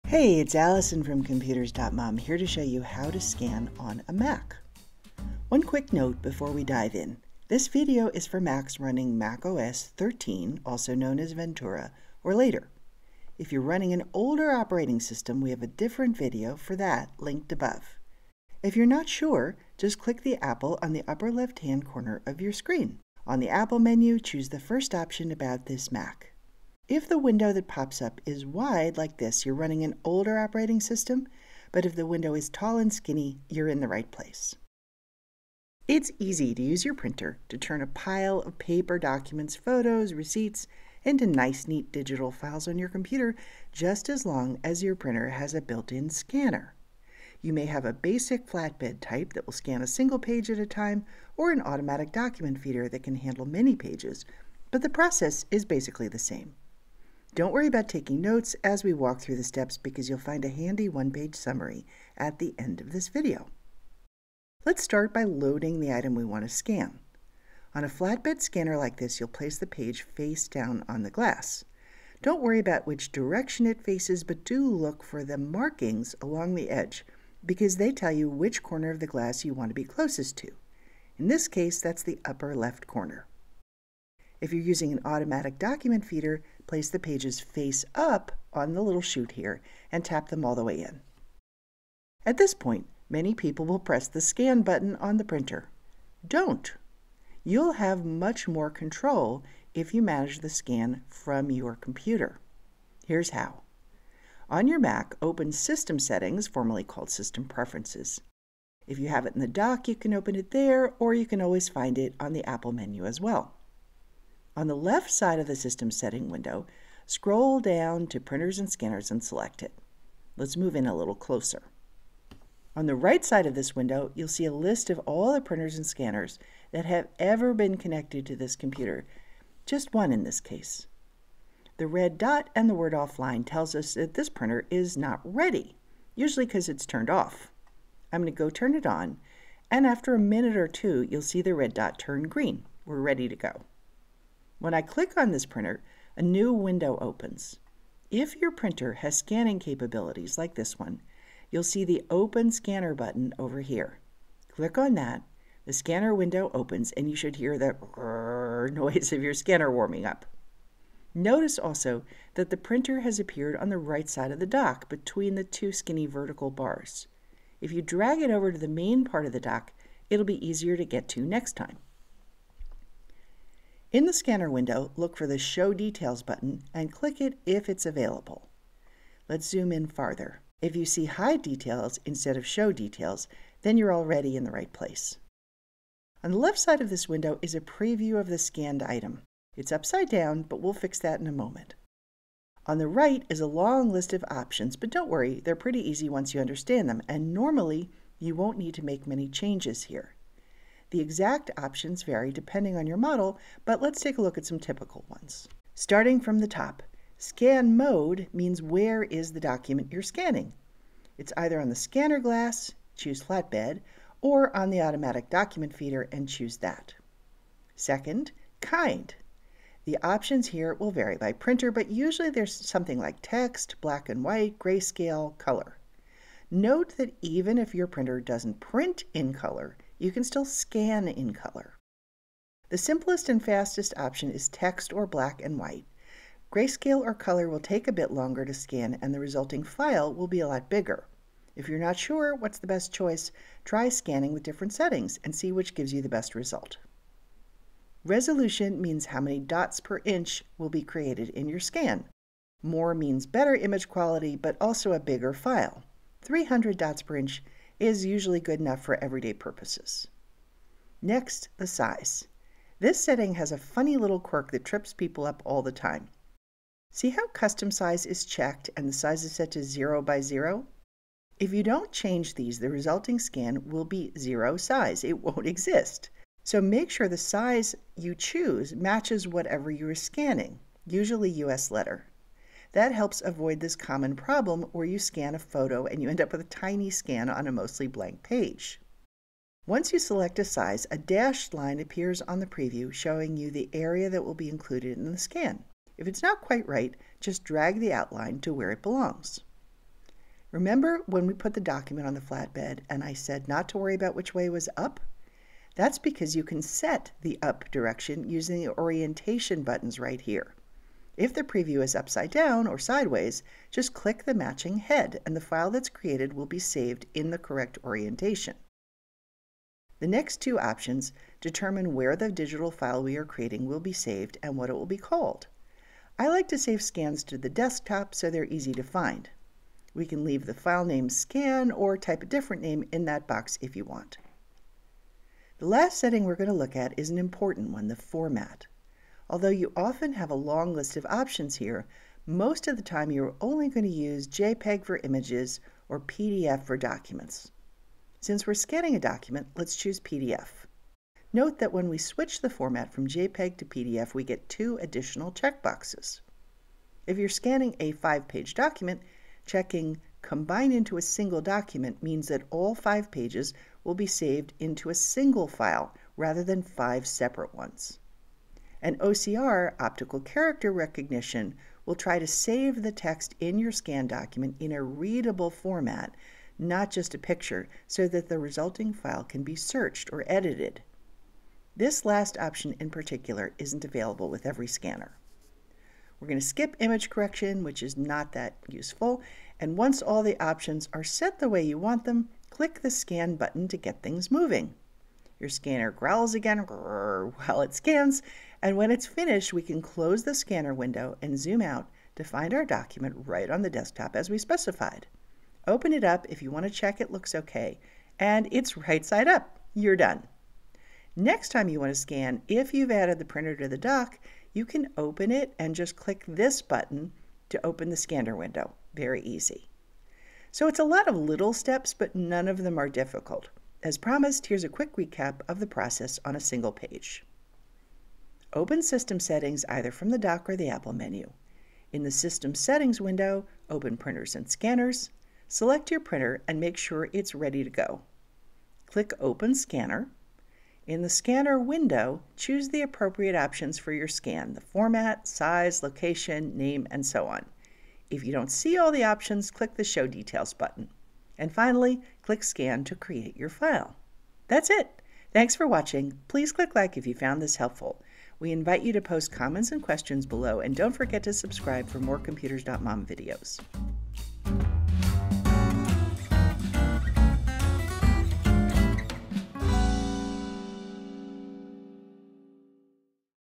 How to Scan on a Mac (MacOS 13+ Ventura and later) [2023]
A practical guide to using the built-in scanning option on your Mac computer to scan from most multifunction printers and scanners, no extra software required.

Walks you through the process in detail with a printable one-page summary of the steps at the end.  Along the way we point out some of the common mistakes people make and how to avoid them!

This video is based on MacOS 13,  Ventura. For Macs running an earlier version of the operating system (Big Sur, Monterey, etc.), please see the earlier version of this video: